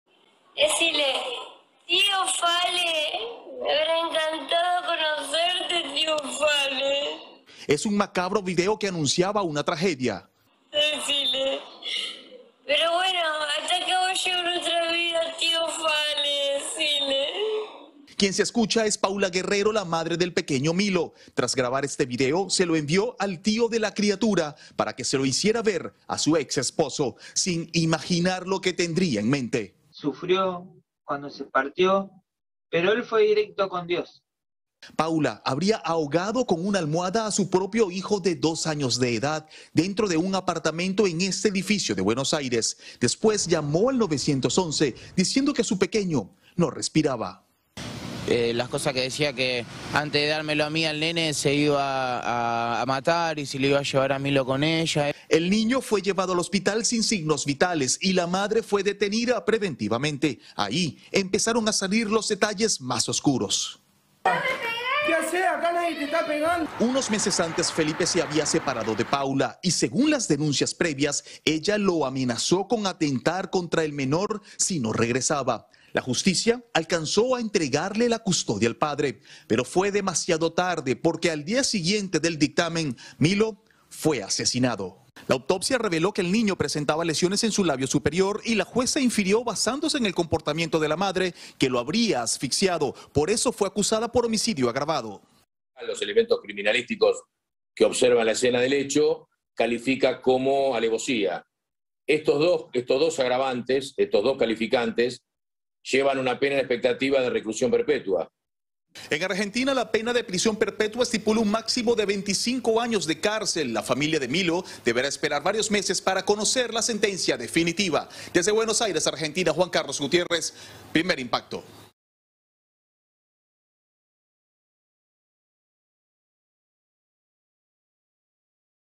Madre es acusada de asesinar con una almohada a su hijo en supuesta venganza contra su exesposo
Paula Guerrero grabó un video para que fuera visto por su expareja. En él aparecía el pequeño y se escucha la voz de ella, quien dice: "hasta aquí llegó nuestra vida". El pequeño, de 2 años, habría muerto ahogado con una almohada dentro de un apartamento en Buenos Aires, Argentina.